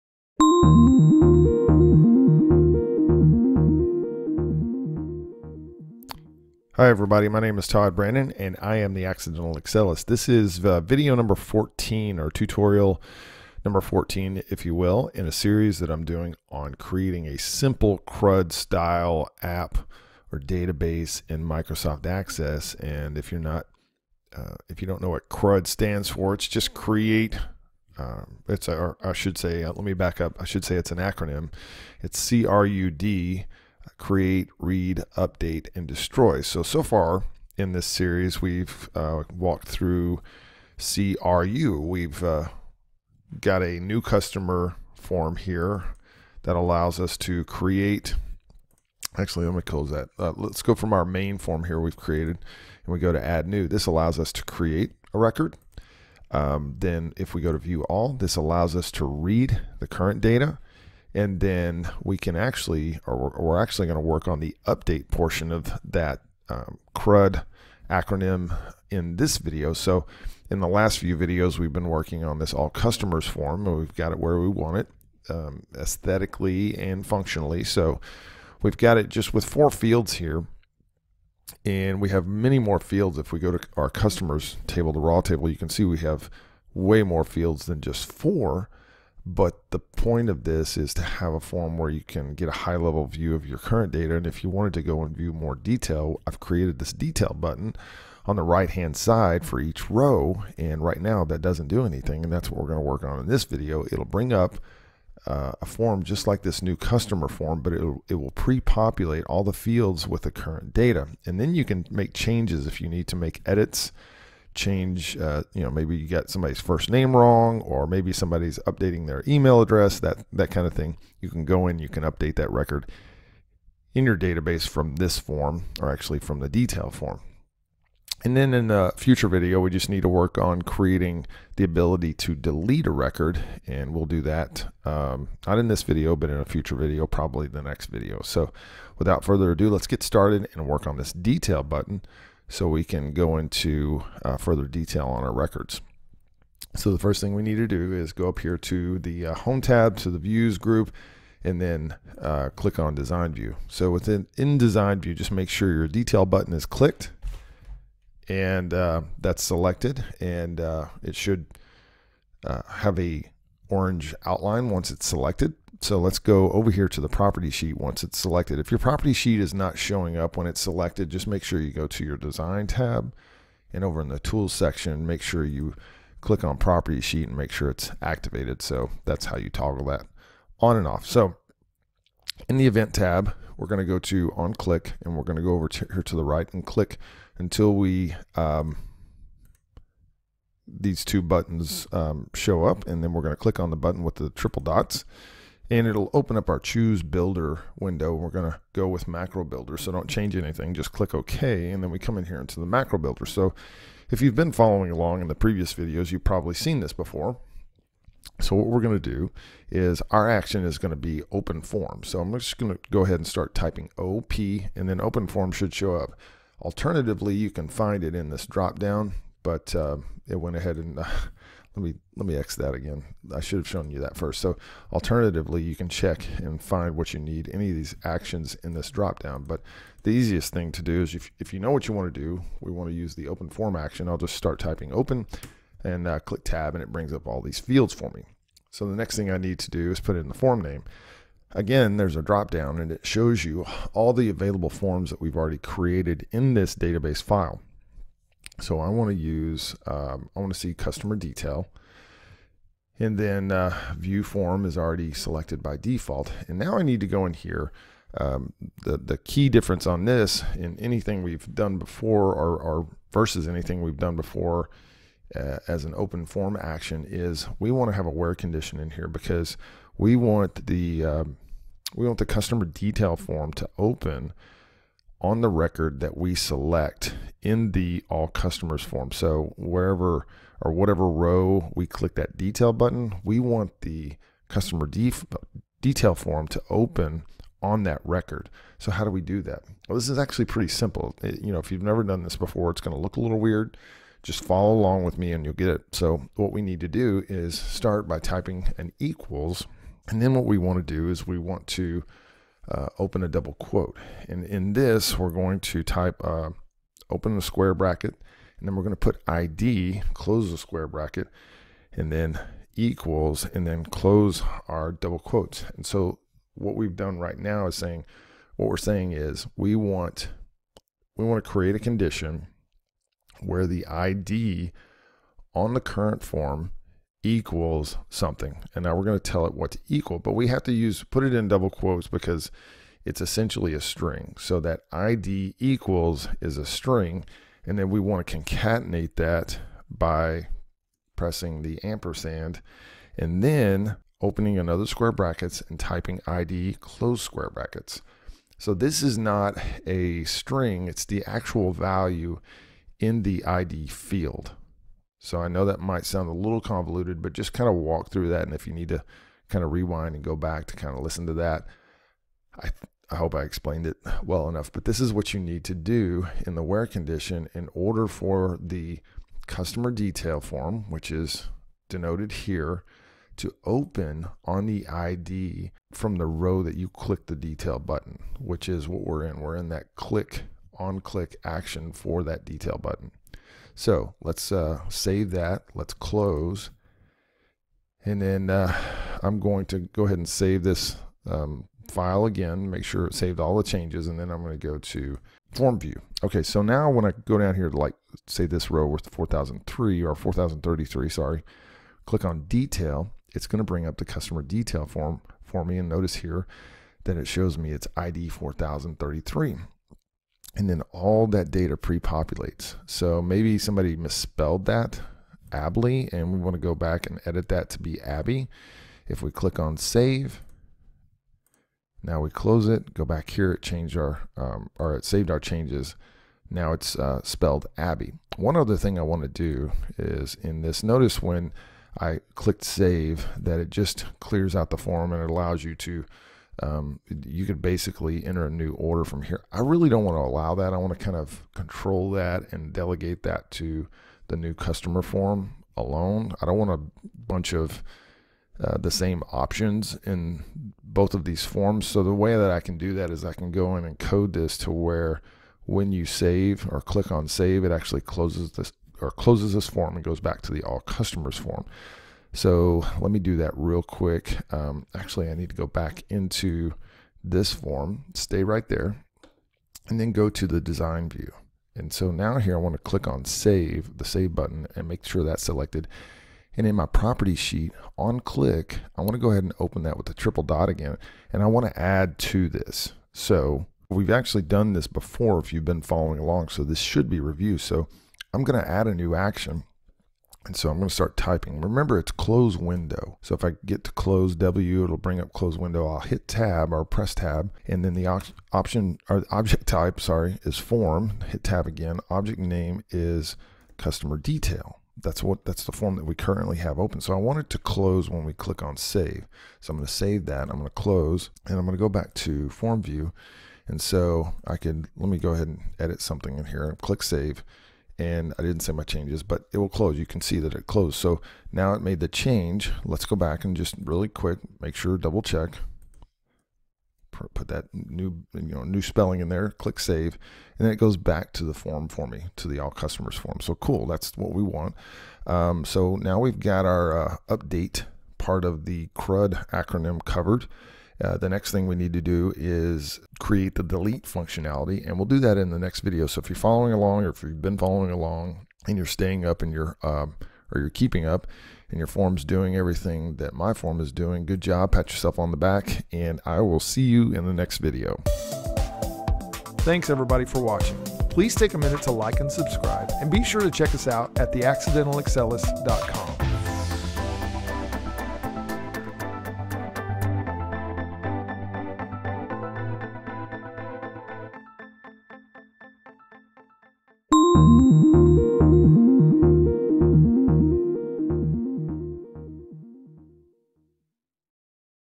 How to Create a Customer Database with forms in Access - 14 of 19
In this video, we create the on click event for the Detail button in the All customers form that will open the Customer Detail form. We can then edit a database record and save the changes.